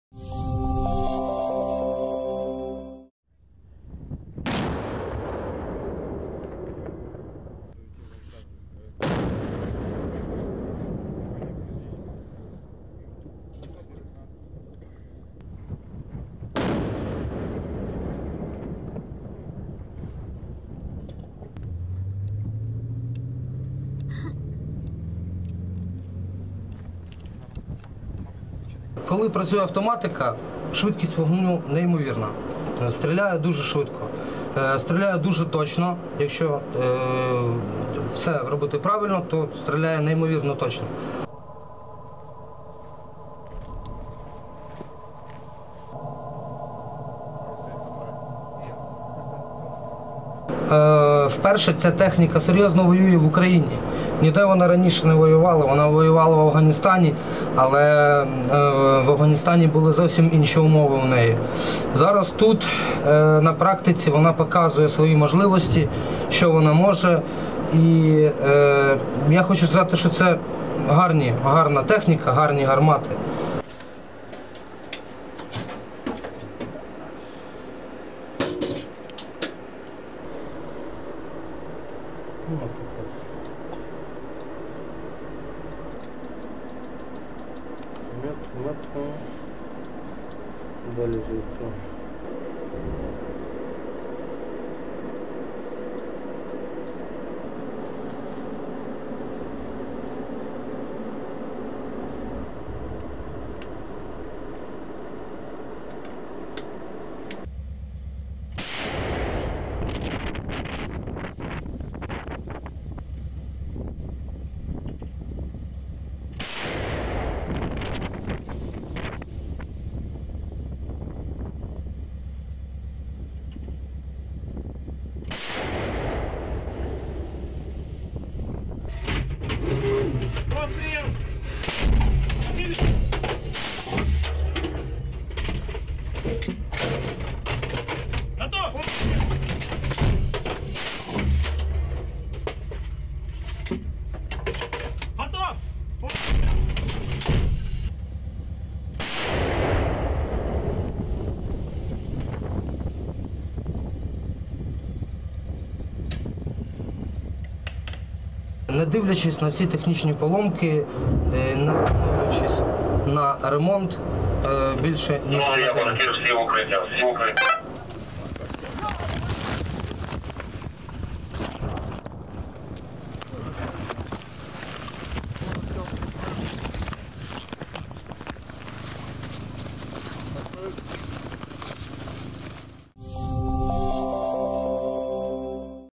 Ukrainian military near Soledar fire German-made howitzer
Ukrainian servicemen fired a German-made PzH 2000 howitzer at Russian positions on Wednesday (January 11) near Soledar.

Despite some technical flaws, the servicemen are satisfied with the PzH 2000.A Ukrainian serviceman who uses the call-sign ‘Pravda’ says their howitzer shot more than 1,300 shells in two months.

“When the automatics work, the fire rate is unbelievable. It shoots very quickly; it shoots very accurately. If you do everything correct, it shoots unbelievably accurately,” said ‘Pravda’.

Russian commanders have targeted Soledar as a platform to attack the nearby city of Bakhmut, which has held out for months against a Russian onslaught.

A victory in Soledar would have symbolic, military and commercial value for Russia after a series of battlefield setbacks in recent months, but its fate was not entirely clear on Wednesday morning.

The Kyiv government said earlier its forces were holding out. The Ukrainian military's morning summary made one mention of Soledar, listing it as one of several towns being shelled in the Donetsk region of the Donbas.

Reuters was unable to verify conditions on the ground.